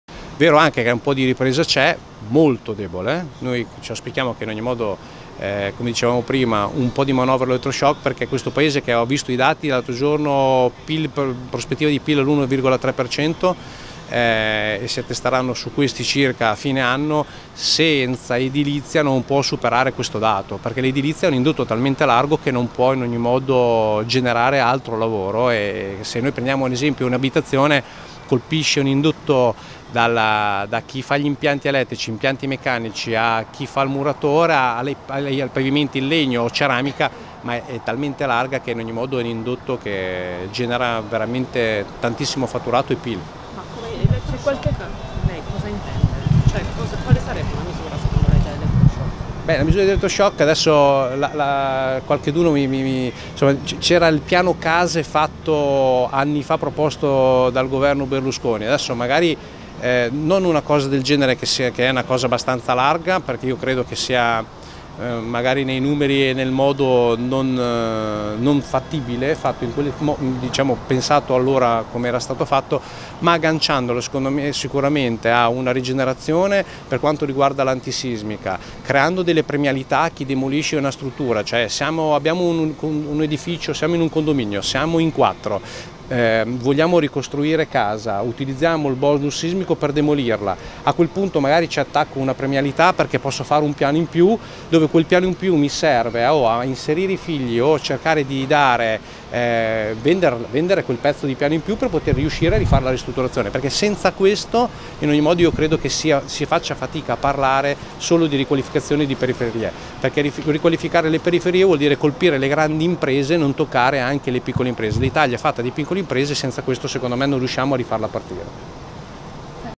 Orsini (Federlegno): "Per far ripartire il Paese serve elettroshock edilizia"
(Agenzia Vista) - Milano, 14 Giugno 2017 - Il Presidente di Federlegno Arredo Emanuele Orsini interpellato a margine della presentazione del primo rapporto sul settore Legno-Arredo.